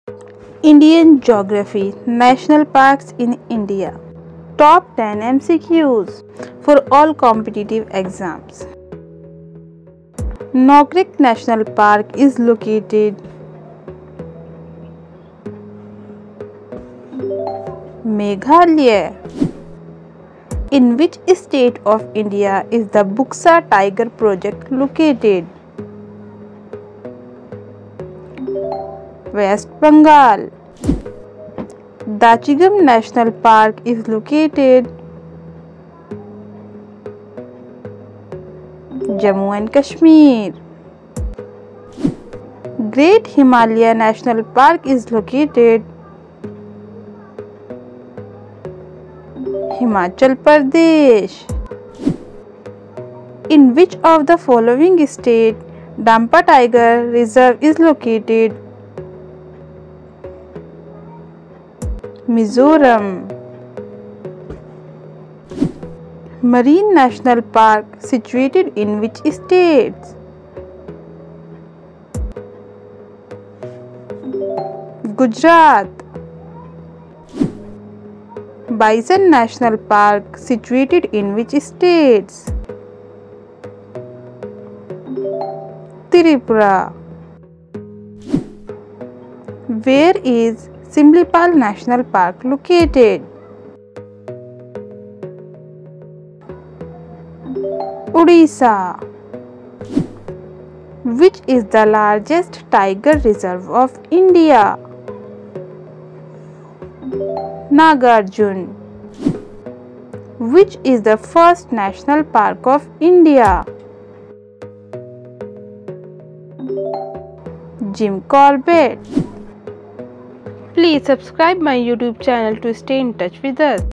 National Parks in India| भारत के राष्ट्रीय उद्यान | Indian Geography| Top 10 MCQs
National Parks in India| भारत के राष्ट्रीय उद्यान | Indian Geography| Top 10 MCQs gk gs@braintreasure895

About this video:- 🙂Dear friends, In this video we will learn National Parks in India Top  10 Most Important Indian Geography Mcqs.
Ummeed Hai Aapko Ye Video Bahut Hi Jyada Pasand Aayegi.🤗

So Much 🤗